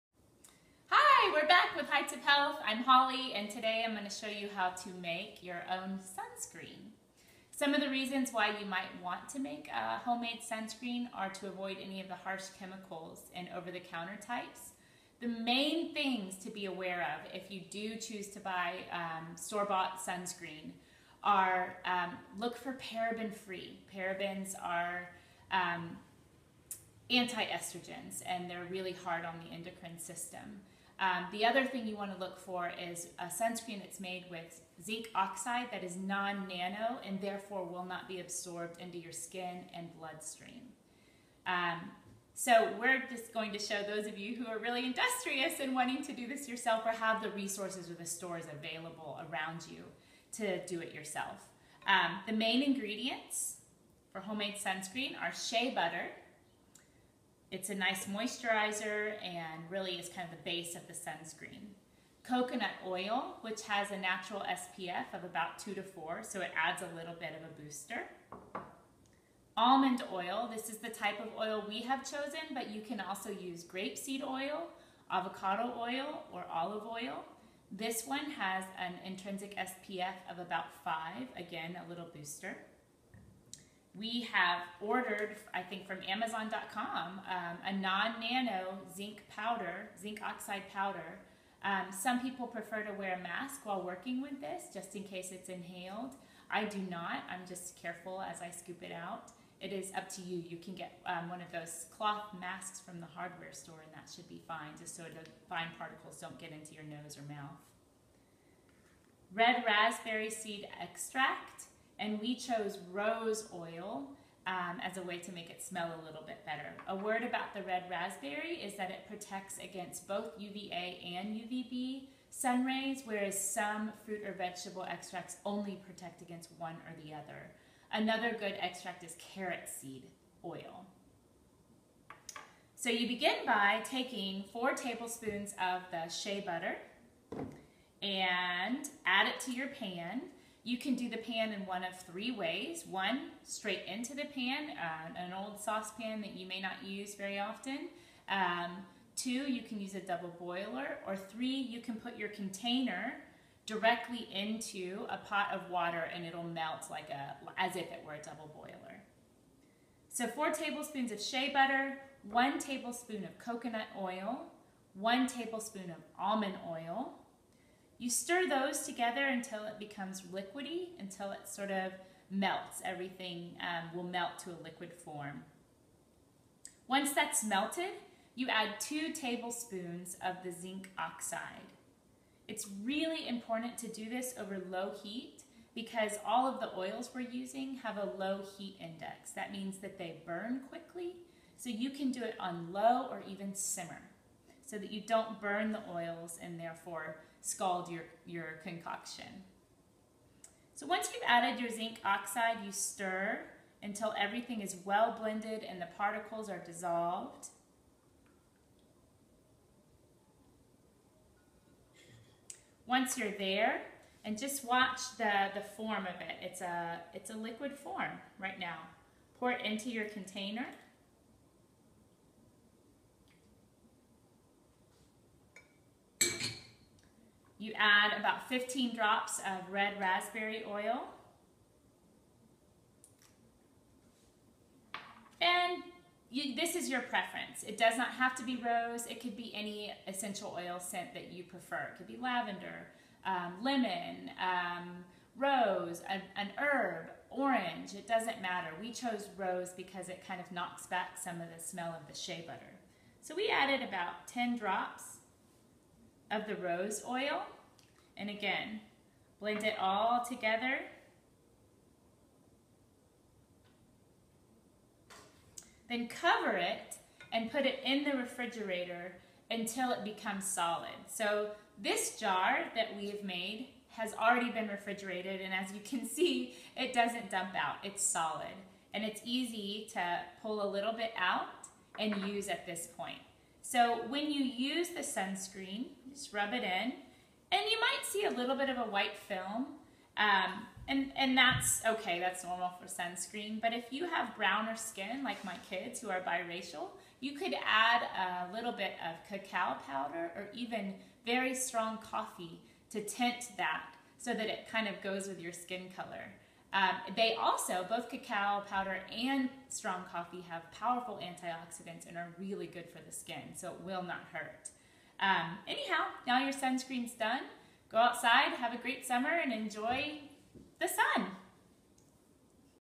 Homemade Sunscreen Demo
Avoid harmful chemicals in traditional sunscreens by making this easy and skin-nurturing all-natural recipe by Holly Hudley.